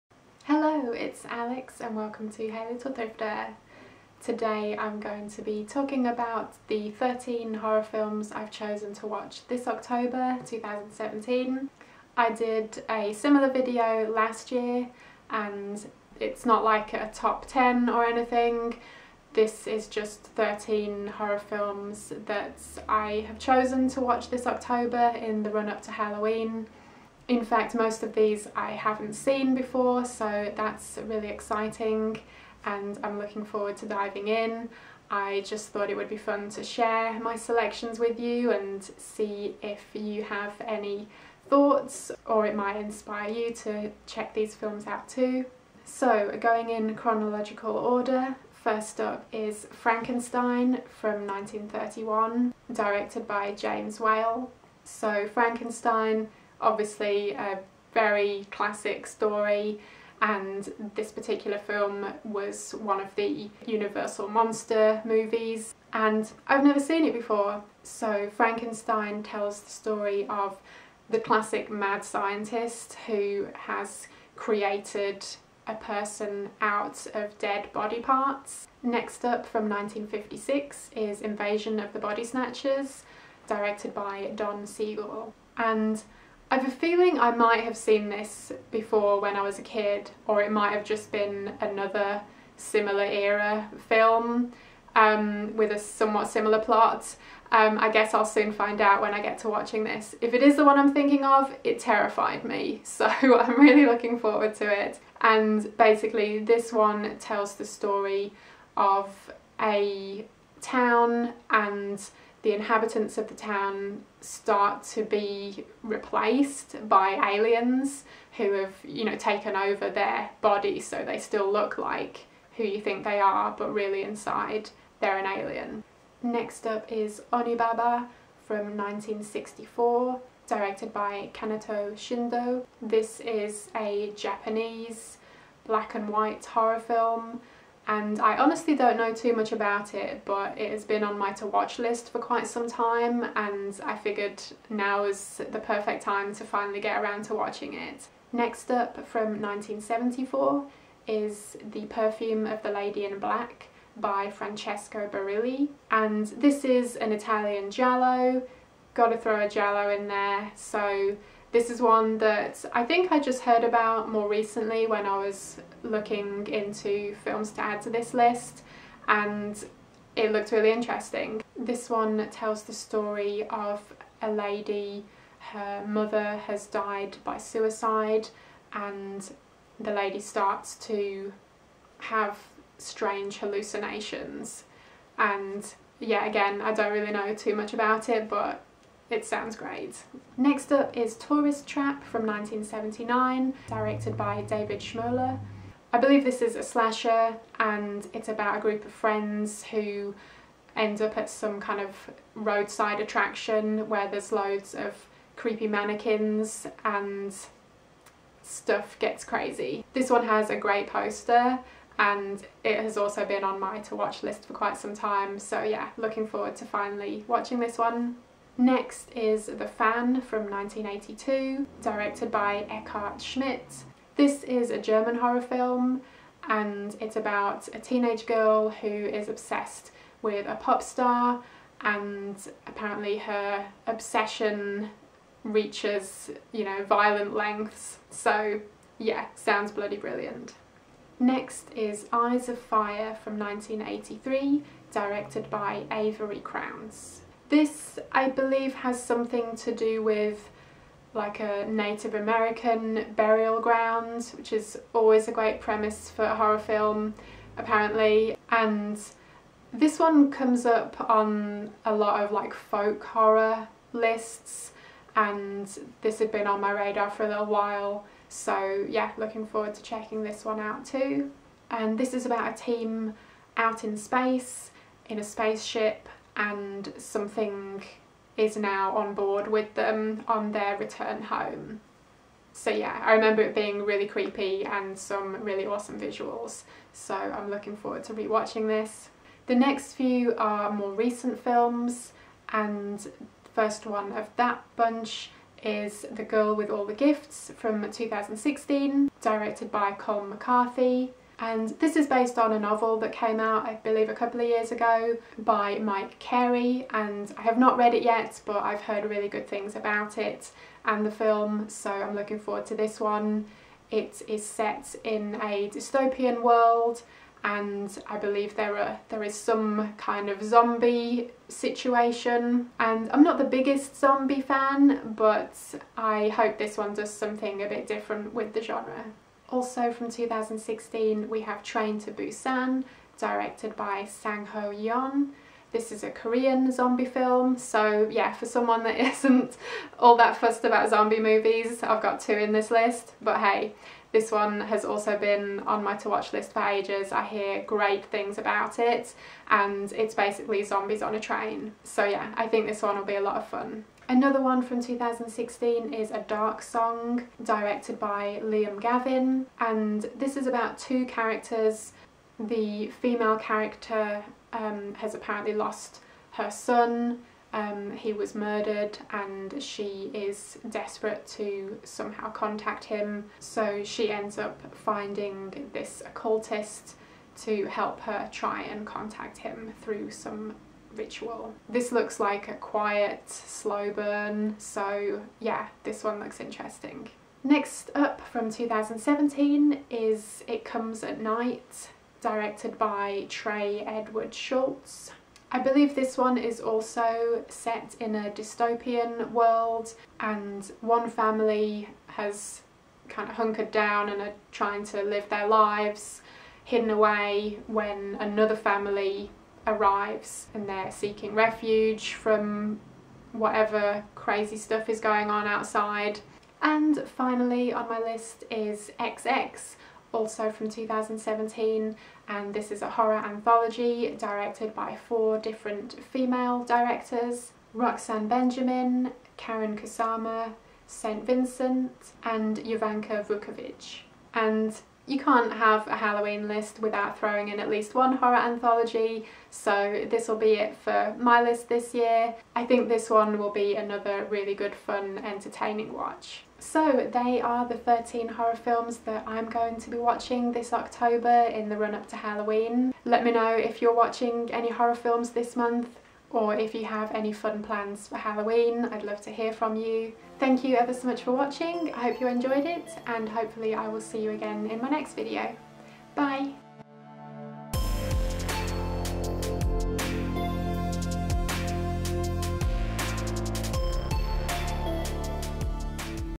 13 Horror Films for October!
Here are 13 horror films that I'll be watching this October in the run up to Halloween :)